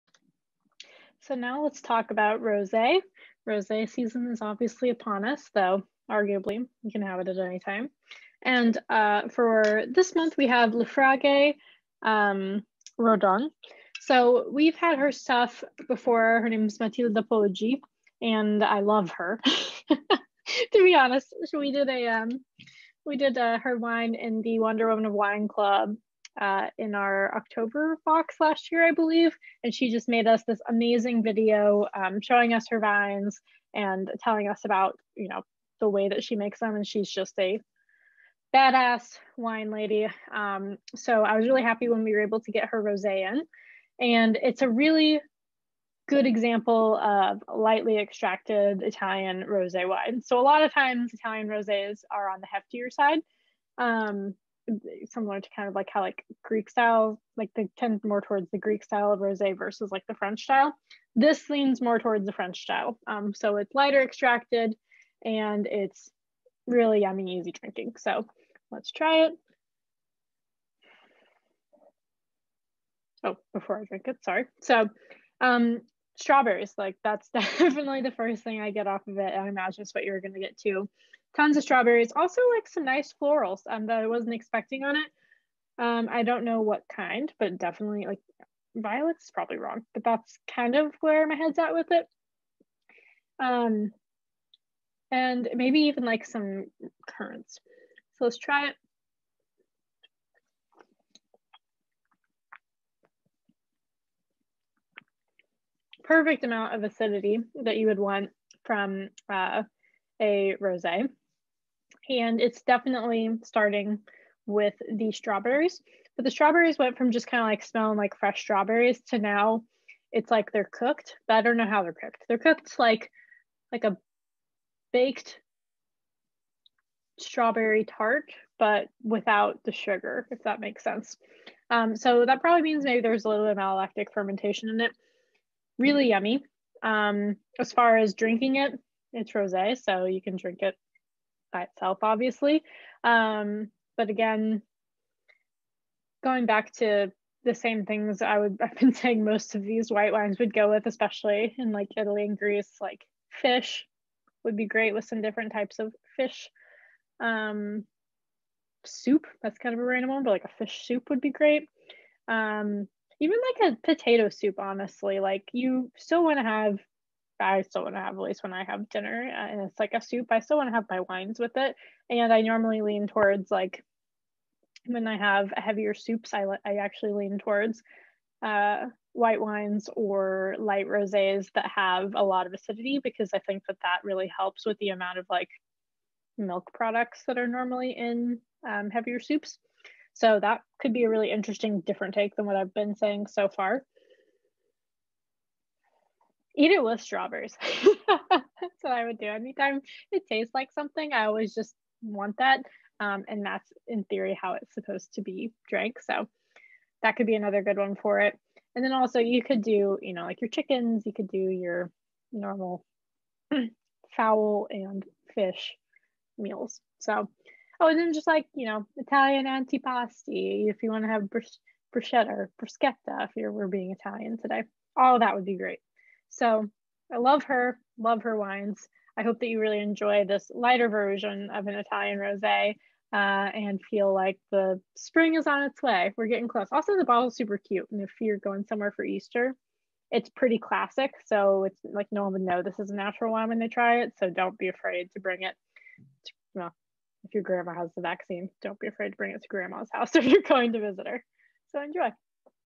Le Fraghe - Rodon
Delicate, bright, and a joy to sip. Scents of herbs, floral, and ripe peach. On the palate, it's tangy and a bit savory. This light rose gives off flavors of wild raspberry, strawberry, hints of white pepper and citrus. Did you know Ròdon translates to 'pink' in Greek?

Type: Rose
Vintage: 2018
Sulfites: Minimal
Alcohol: 12.5%
Varietal: Corvina, Rondinella
Production: Organic
Style: Glou Glou